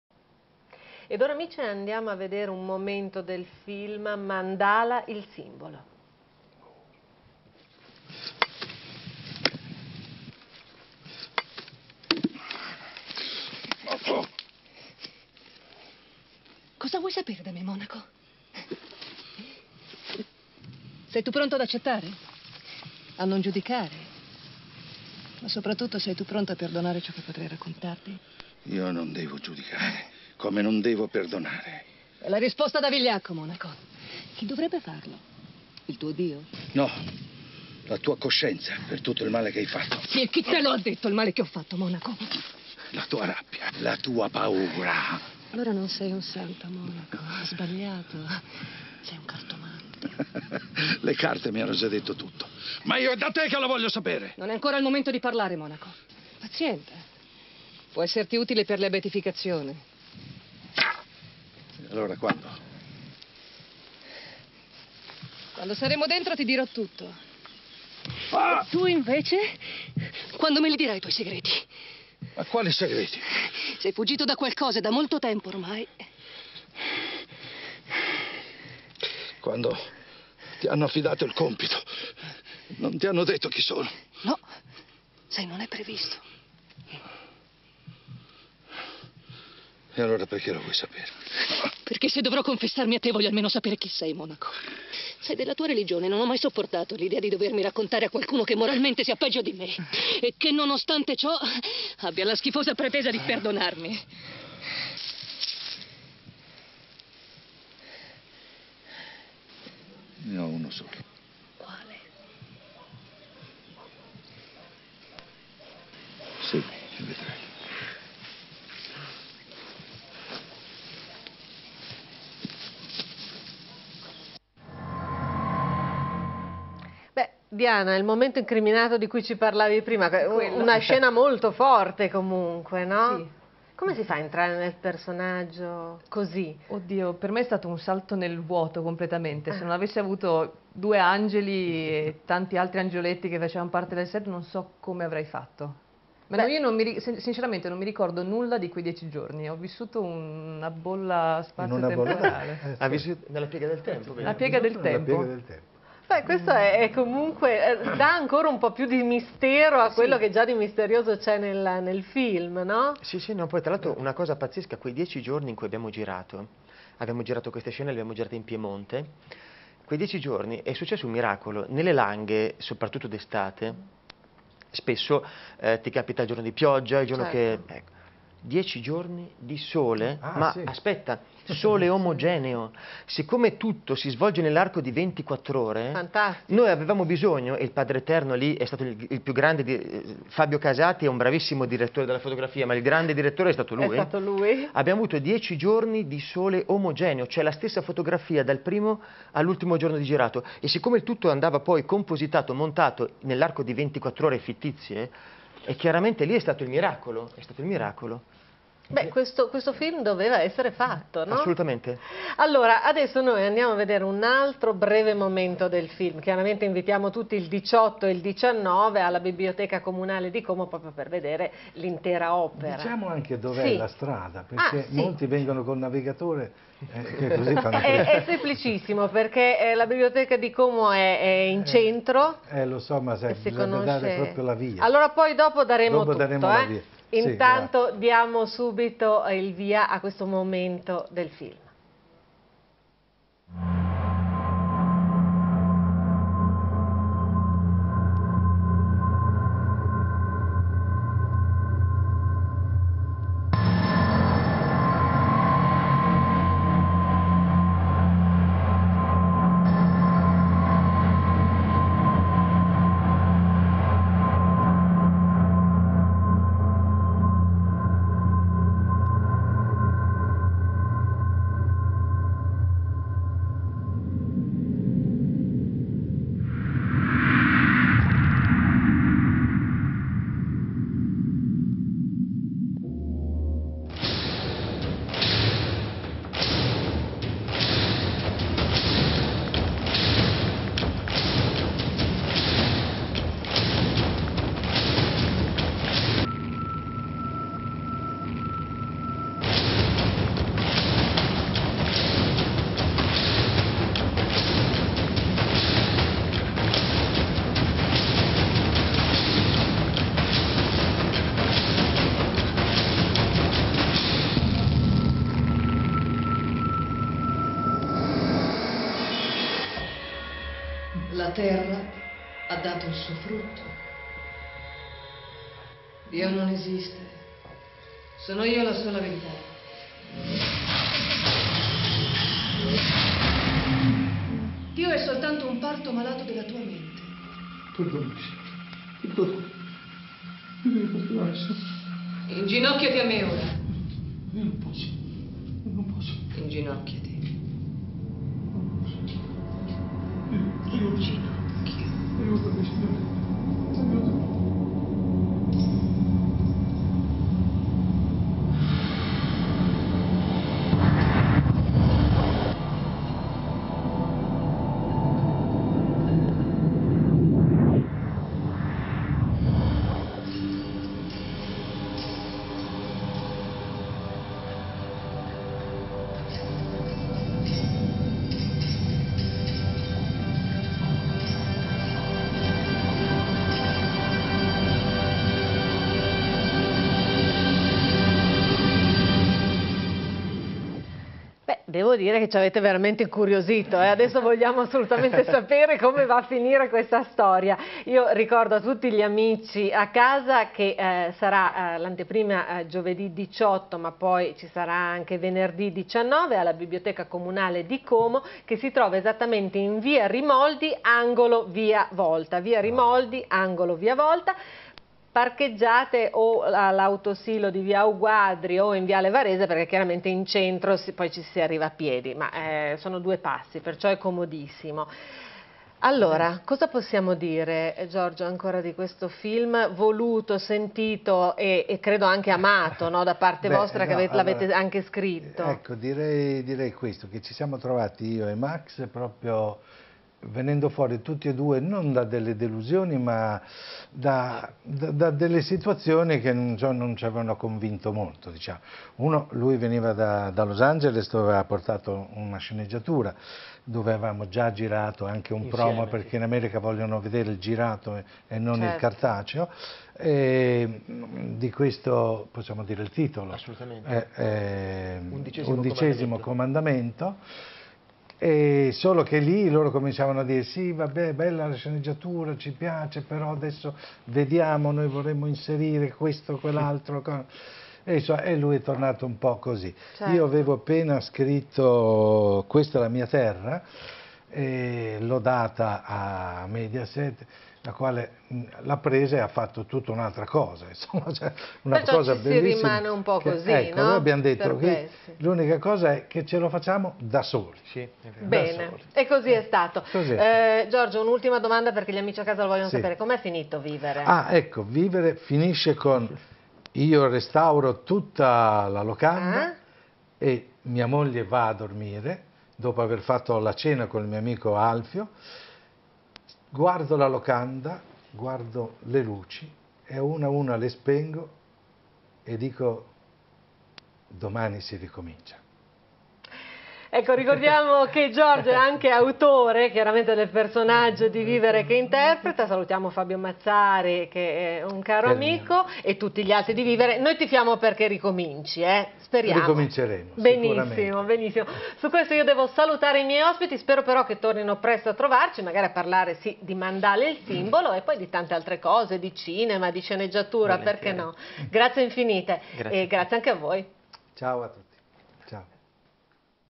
Max Leonida - ZeroTreUno TV interview (part 3)
Prima parte dell'intervista al regista e autore Max Leonida e agli attori protagonisti Giorgio Biavati e Diana Manea in occasione della presentazione del film MANDALA a Como.